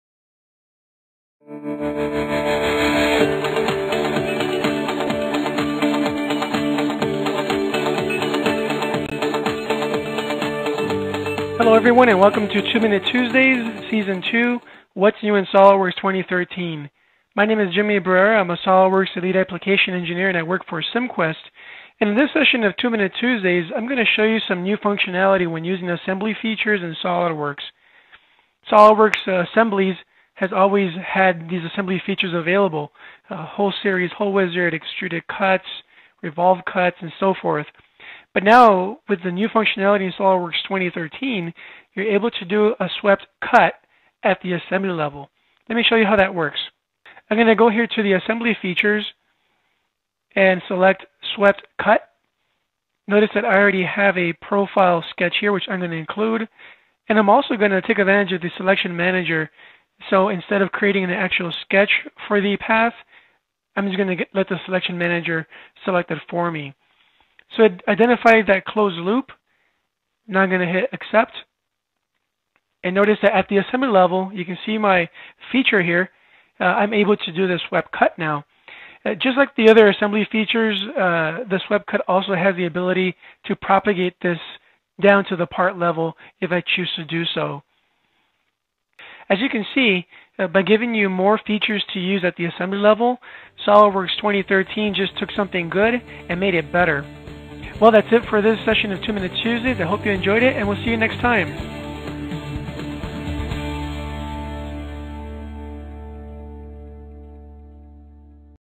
New Assembly Feature: Swept Cut 2 Minute Tuesday - What's New in SolidWorks 2013 by Cimquest
SolidWorks 2013 continues to improve its interface by allowing the swept cut feature to be used in assembly mode making your job a little simpler.